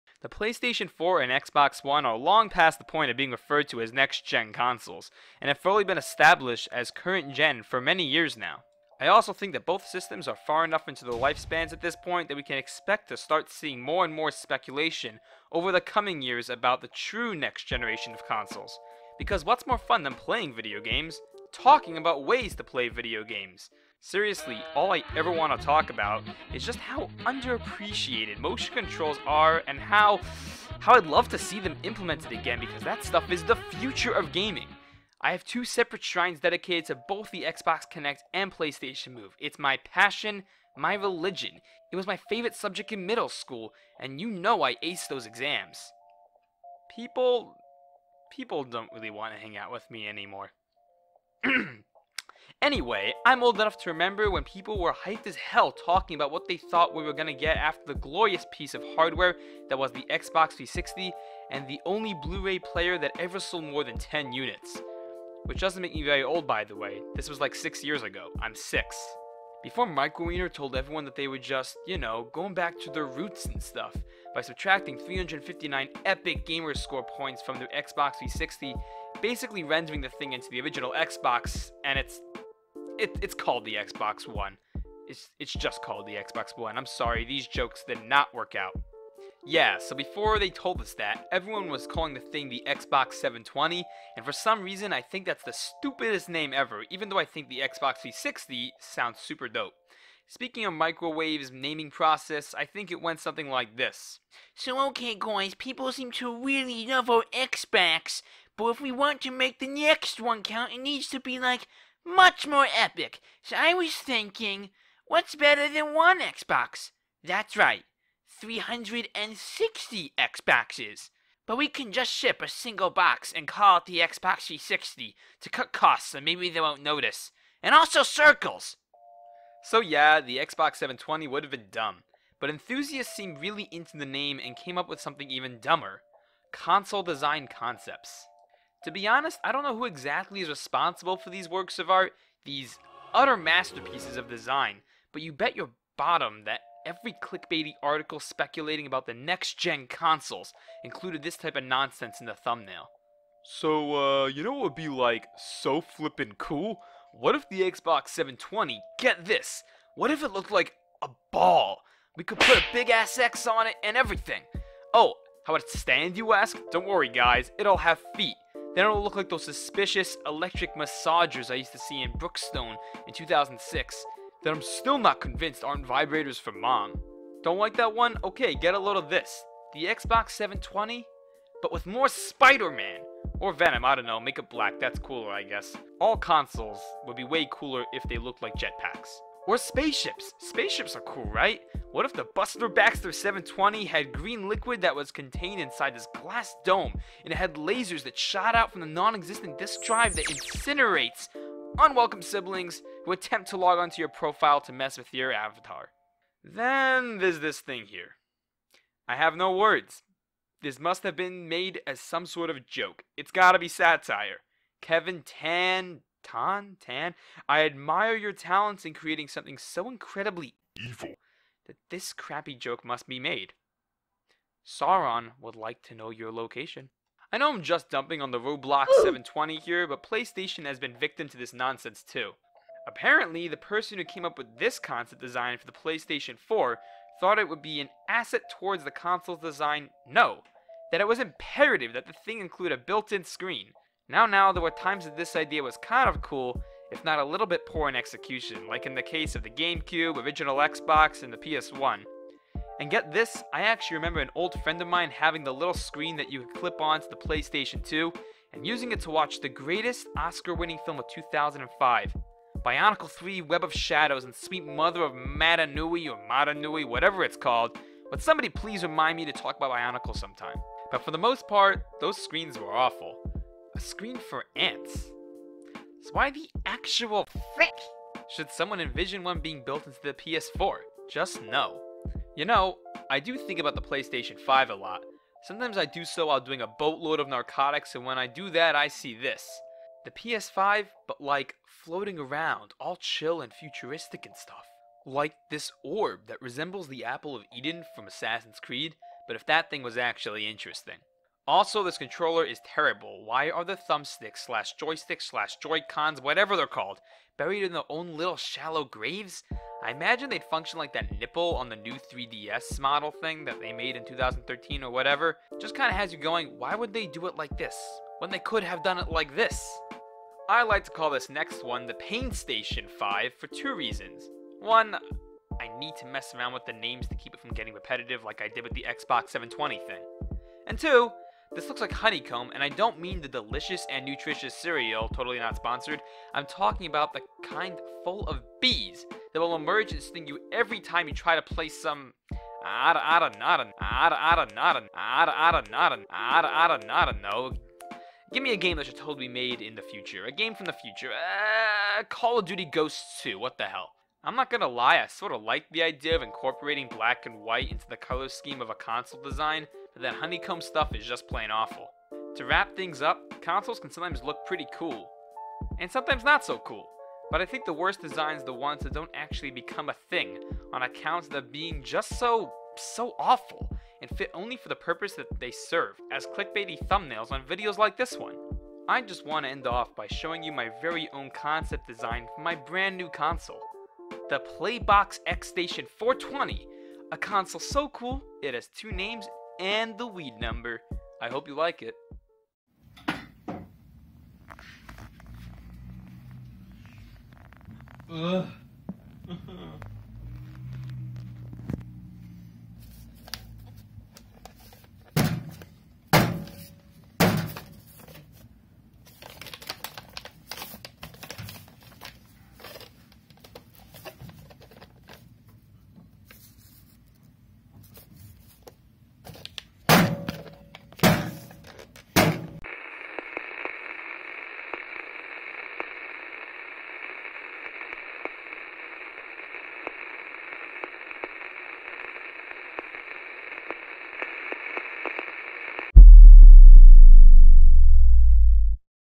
Stupid Console Concepts
In this vee-dee-oh, I take a look at some of my favorite, not-so-favorable game console design concepts, and poorly deliver some bad jokes.